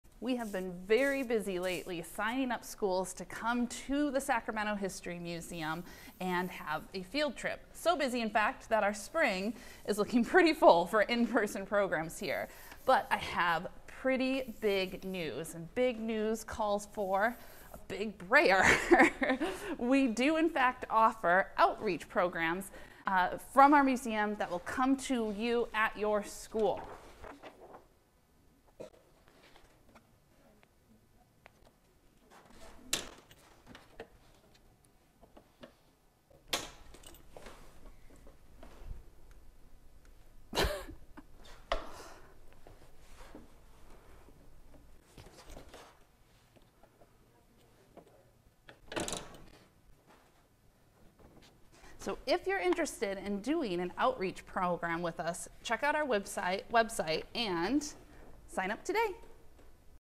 Letterpress printing with large type and a large brayer to get the word out for our school programs
The field trip season is underway at the Sacramento History Museum but our in-person school program offerings are filling up for the spring!

In this video, Susan letterpress prints with our largest wood type and discusses that we have outreach programs. We have recognized that many schools in the Sacramento area cannot visit our museum in person due to transportation costs, bus availability, and other barriers. However, let us bring our museum to the classroom instead!

Want to learn more about our outreach programs and our other school programs?